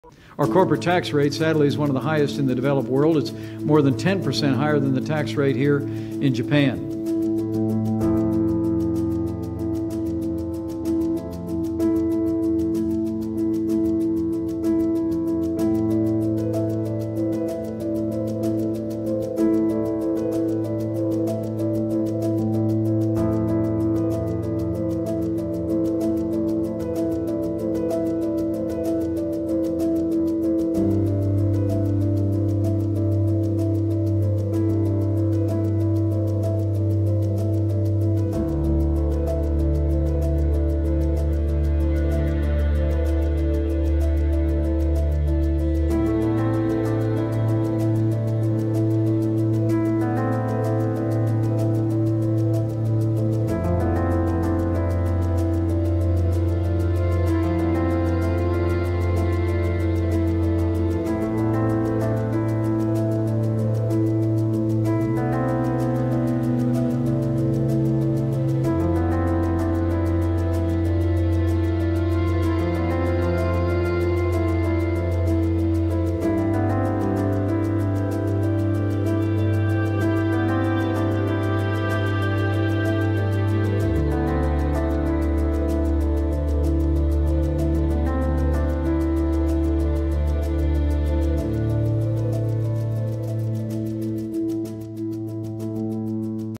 Myths and facts about the corporate tax rate
It's often said that businesses in America pay higher taxes than anywhere else. But what U.S. corporations actually pay varies widely. In many cases, it's nothing.